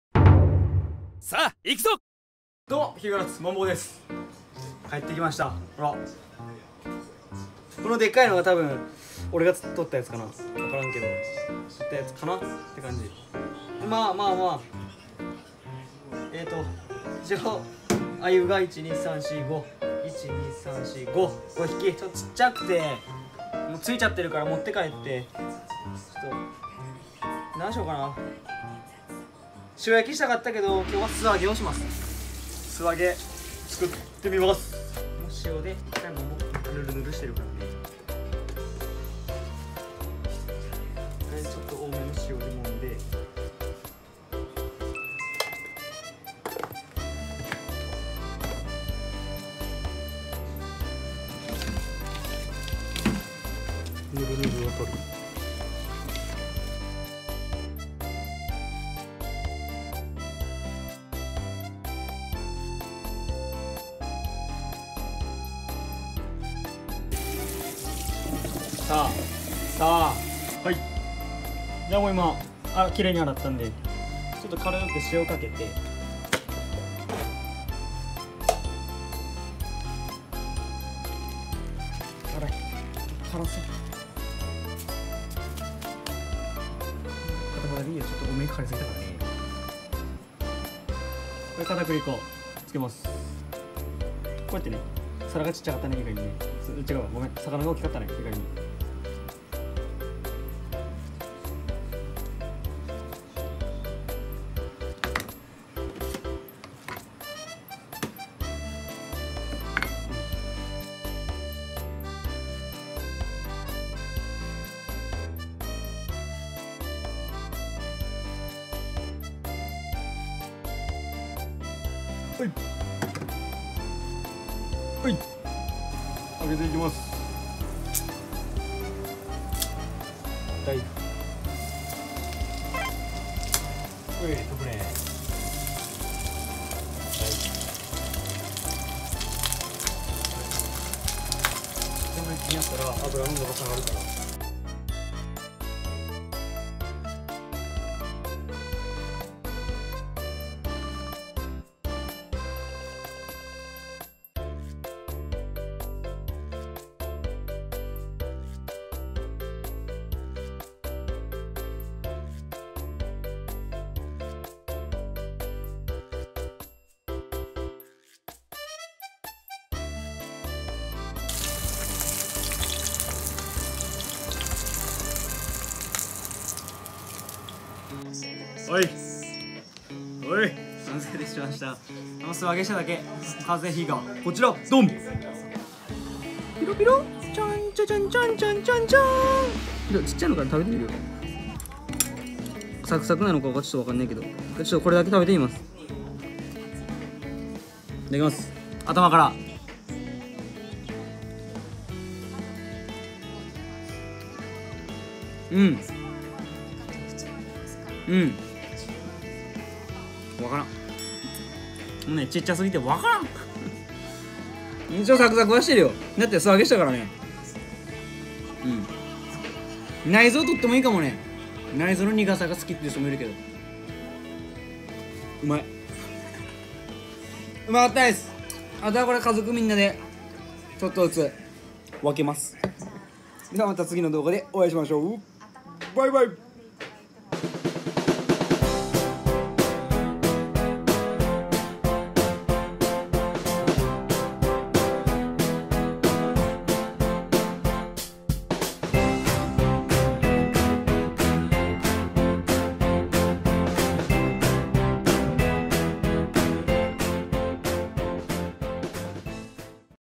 【料理】鮎は素揚げっしょ!?突きたての天然鮎を油にダイブさせて食べてみた!!【鮎料理】【宮崎釣り】【渓流釣り】
◾️リクエストフォームを開設しました。下記のリンクからどしどしリクエストお願いします。

編集：リーダーしょうご